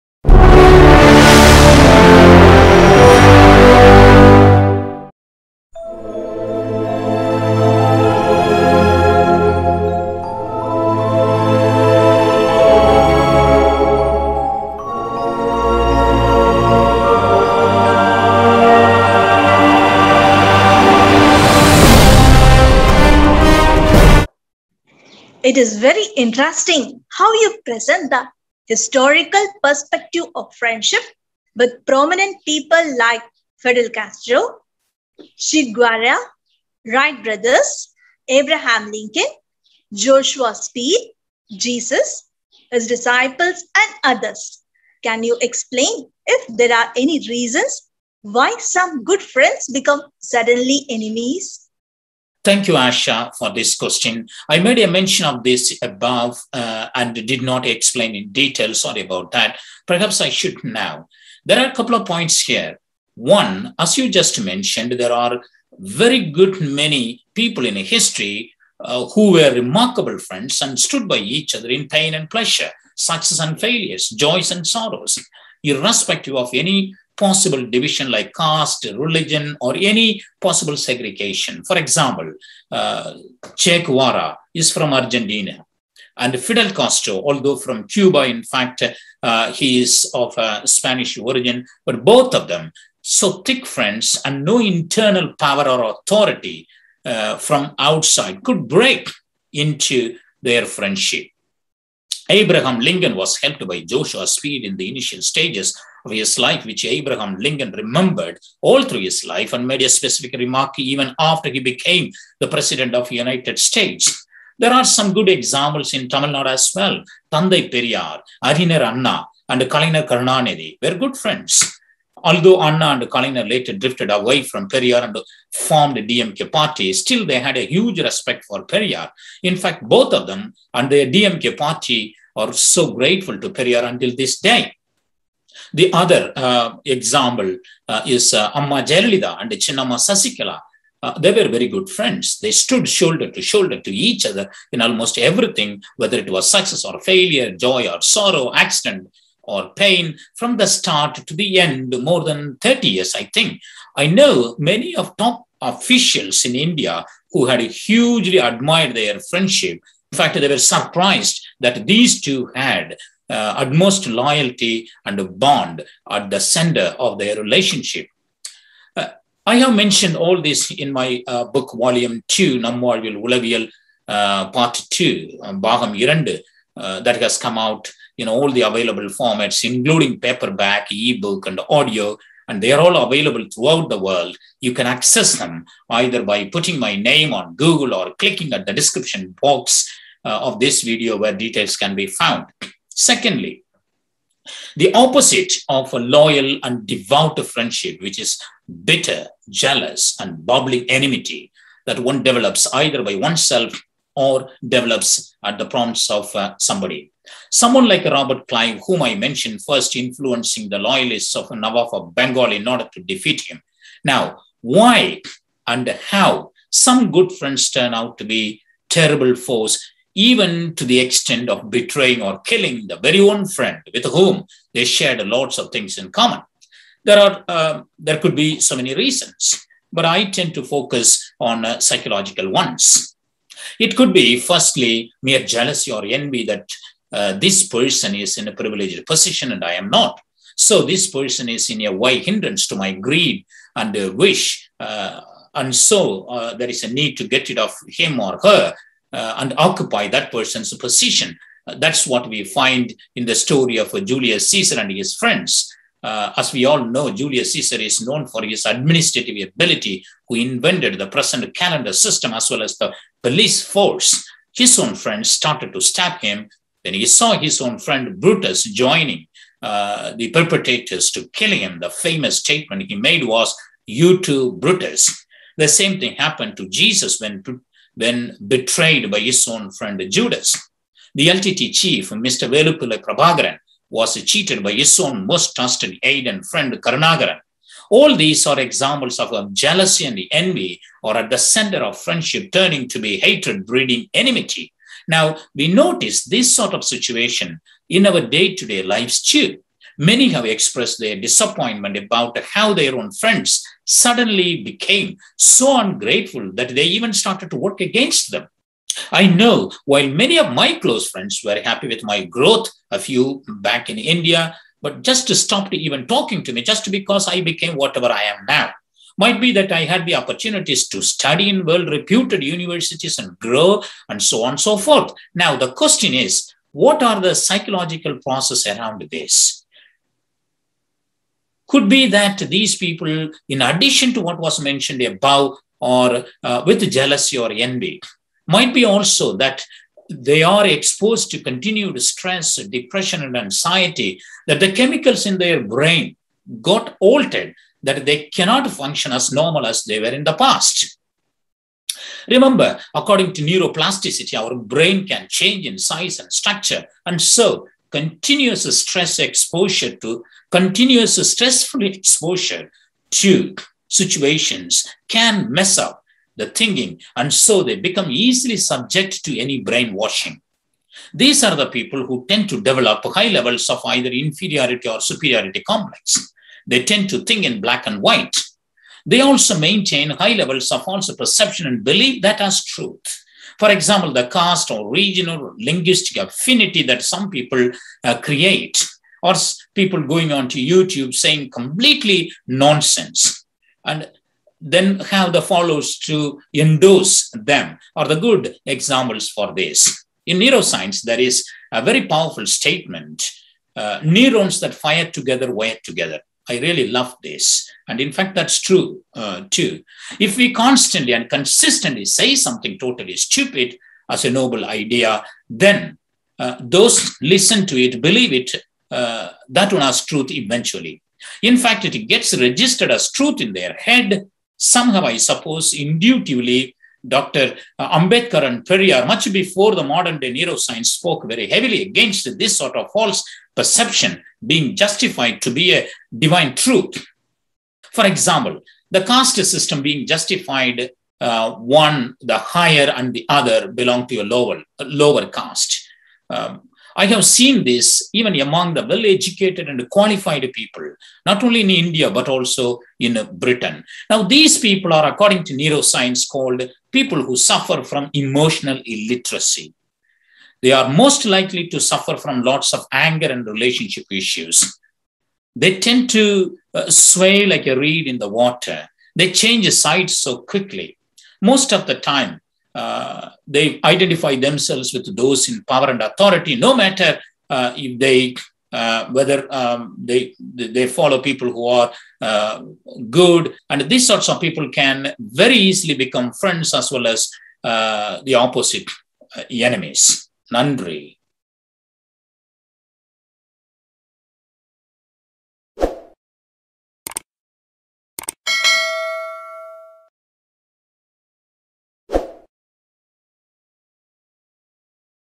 2.2 Can Tamils have lasting friendships? Periyar|Kalaignar|Jayalalitha|Sasikala|DMK|Neuroplasticity.
This is a talk given by London based Psychologist Kolakkudiyar on the topic 'Tamils' Friendship: A scientific view'. This fantastic speech contains critical examination of Tamils' way of life all over the world. This is presented with cutting edge research and fun. Please do not miss.

Links to Kolakkudiyar's Books worldwide are below.